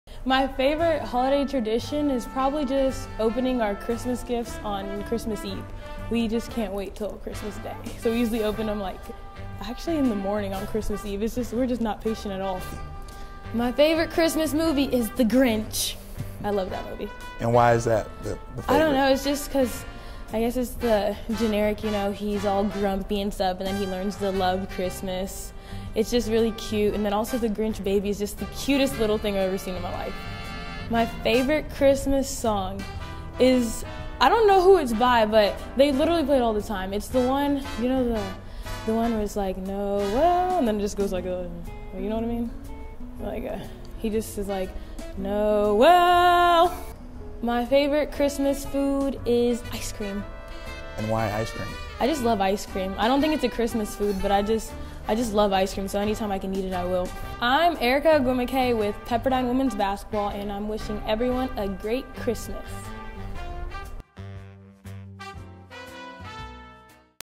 Pepperdine Women's Basketball Holiday Edition: Erica Ogwumike
Hear from Pepperdine Women's Basketball freshman guard Erica Ogwumike and her favorite part about the holiday season.